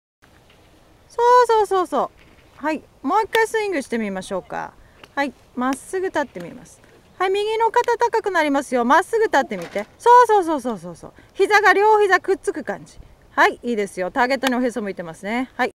kazkuko ideal finish
フィニッシュで右肩が落ちて、フィニッシュまで振り切れない傾向があります。右肩が左肩より高くなる意識で振り切りましょう。体がターゲットに向くまで振り切れます。